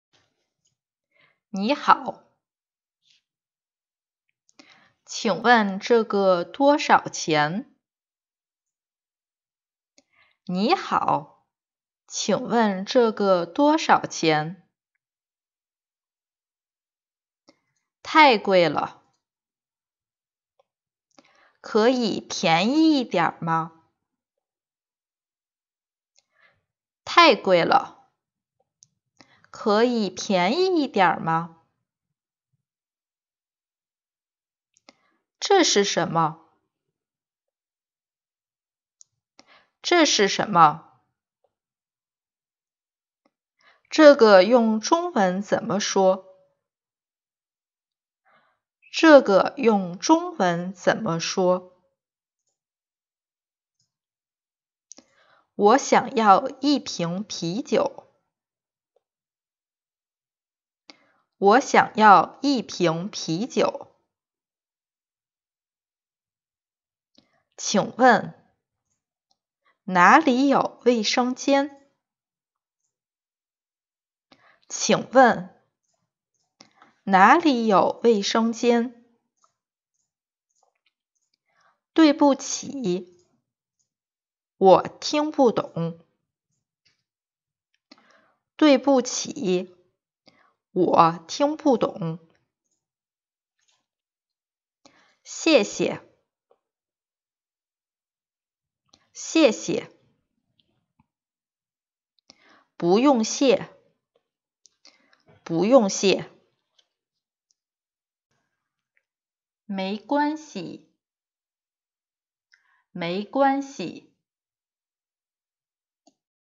10 phrases super importantes à apprendre en chinois
Si vous voulez voyager en Chine, voici les 10 phrases que vous devez apprendre comme:
Demander un prix, et demander un bon prix;
Chercher des toilettes;
Demander une bière etc.

Meina